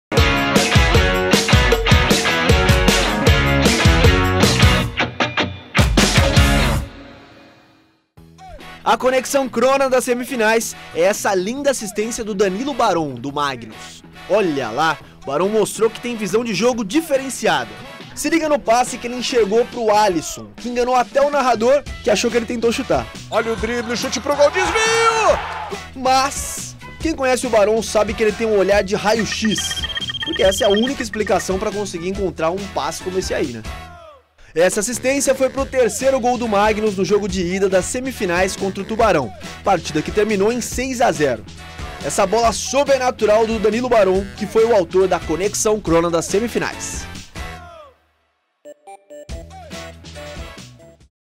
LNF2020 - Assistência das Semifinais
Danilo Baron, do Magnus, deu a Assistência mais bonita das Semifinais da LNF2020